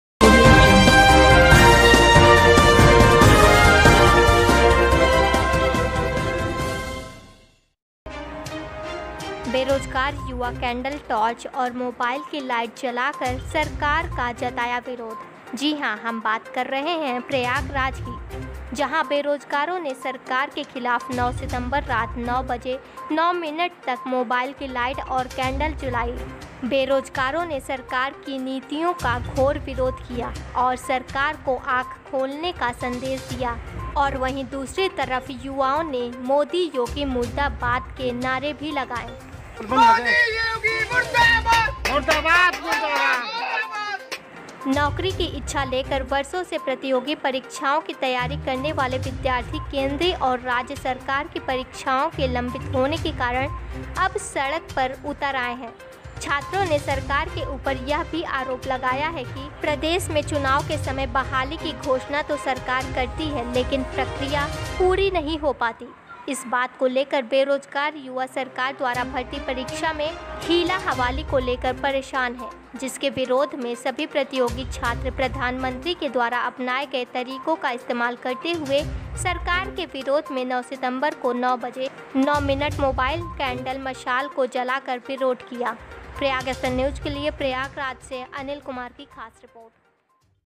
बेरोजगार युवा कैंडल टॉर्च और मोबाइल की लाइट जलाकर सरकार का जताया का विरोध
प्रयाग एक्सप्रेस न्यूज़ रिपोर्ट अनिल कुमार